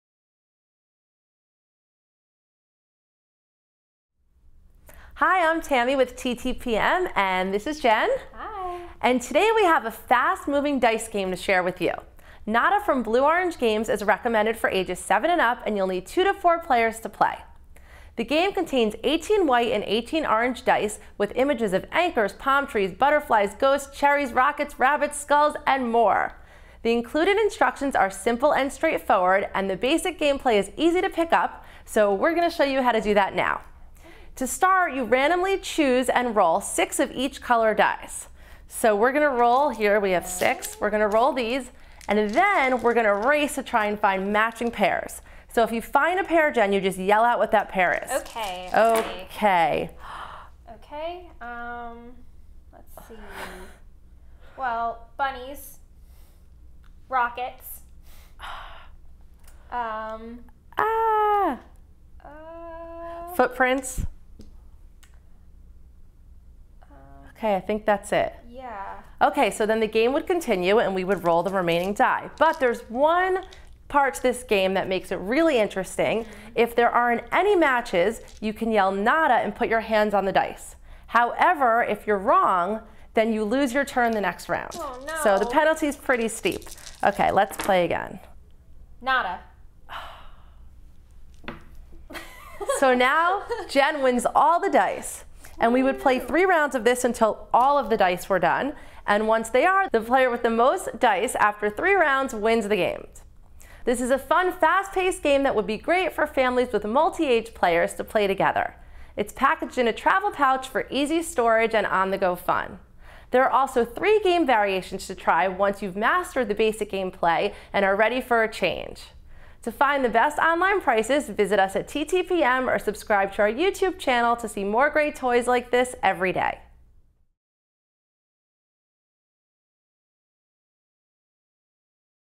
Nada from Blue Orange Games
Nada is a dice game where two to four players roll dice and try to find matching images to win the dice. For full review and shopping info

Product Info:
Nada is a dice game where two to four players roll dice and try to find matching images to win the dice. The game comes with 18 white and 18 orange dice with images of anchors, palm trees, butterflies, ghosts, cherries, rockets, rabbits, skulls, and more. You randomly choose and roll six of each color dice. Then players race to find matching pairs that include at least one from each color and shout them out. The first player to shout the match wins the dice. Play continues with the remaining dice until all the dice have been collected. If during a roll there are no matches, the first player to shout "Nada!" and put his or her hand on the dice wins them all. If that player is wrong, though, the penalty is steep, and he or she sits out until the next round. This game is played in three rounds using 12 new dice each time, and the player at the end of the three rounds with the most dice wins.